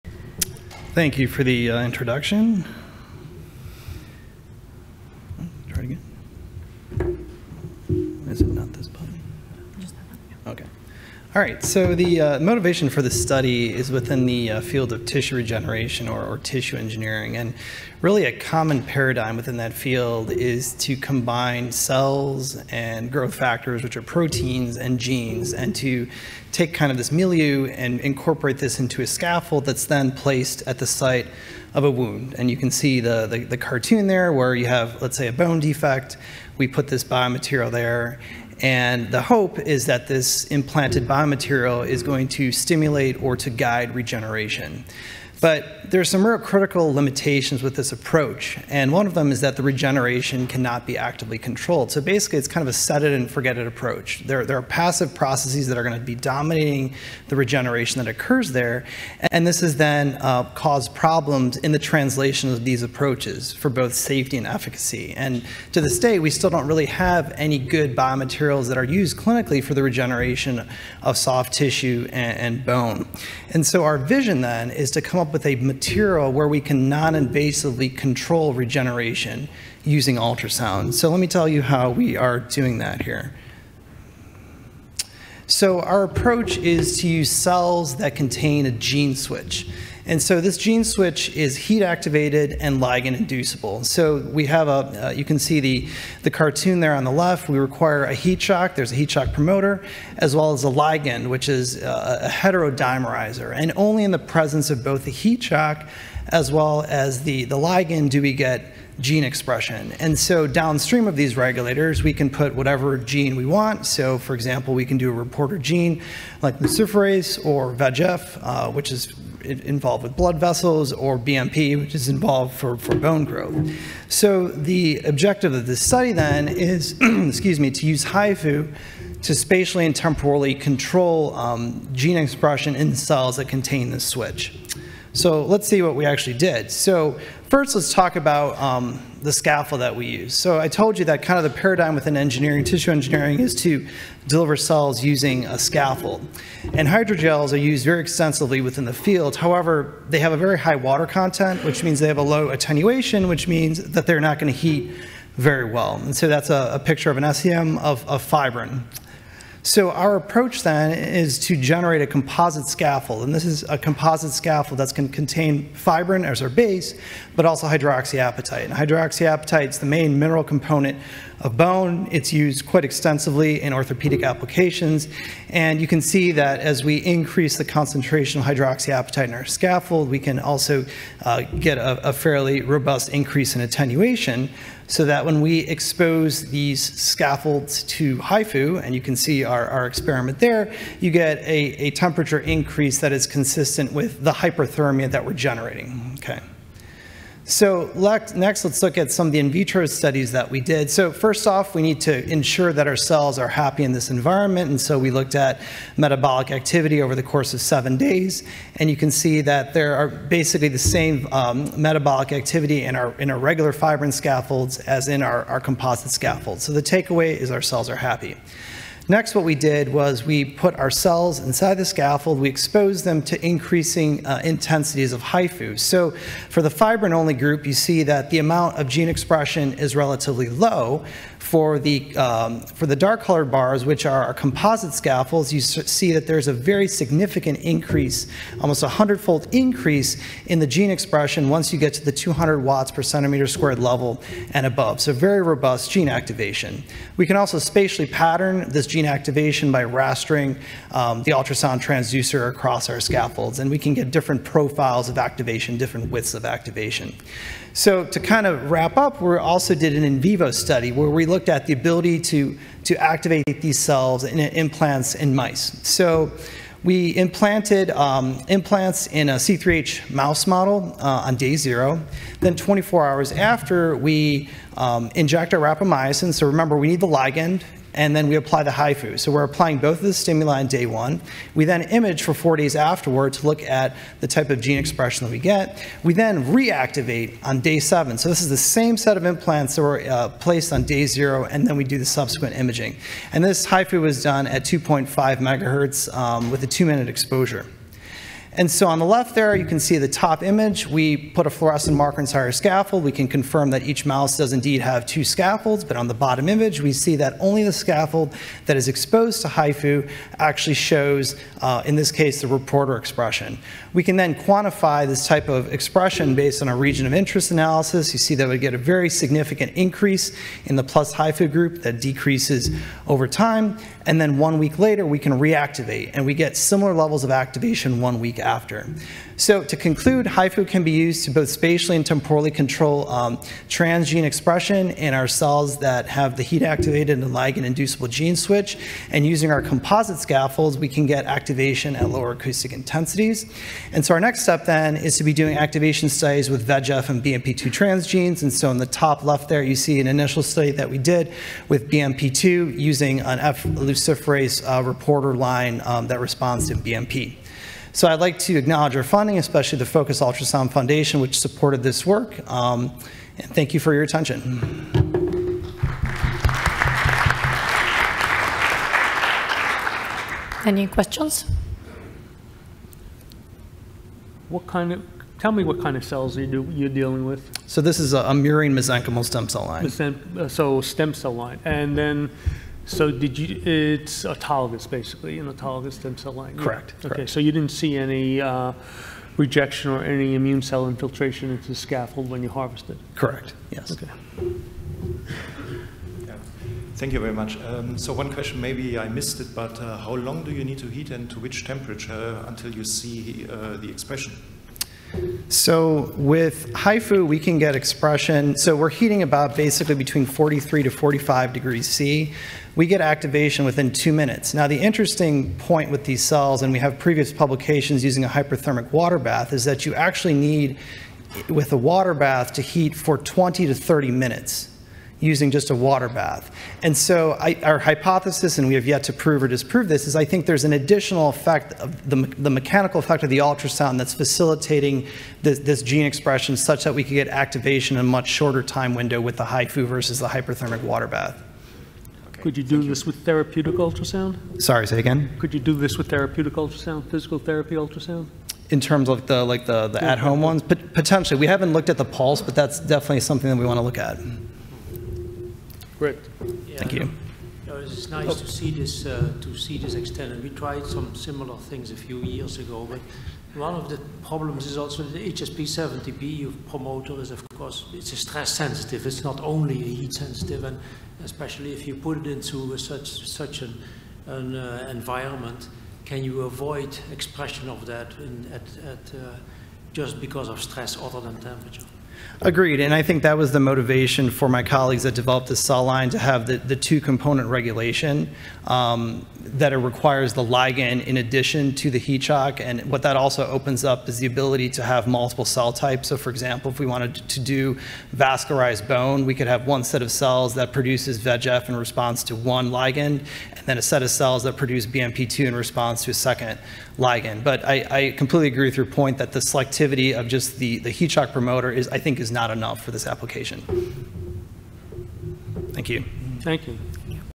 Mario Fabiilli - Spatiotemporal control of transgene expression using hyperthermia by HIFU (2018)
Non-invasive, spatiotemporal control of transgene expression using hyperthermia generated by high intensity focused ultrasound